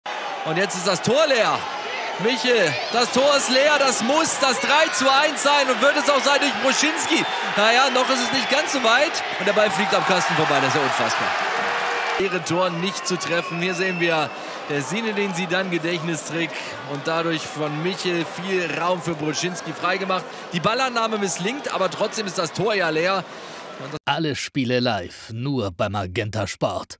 Wenn du auf deinem Schein Handicap Dortmund hast 😫 | 3. Liga | MAGENTA SPORT
Unglaublicher Fehlschuss von Moritz Broschinski! Am Ende kann sich der BVB II dennoch mit 2:1 gegen Zwickau durchsetzen.

Alle Spiele der 3. Liga live bei MagentaSport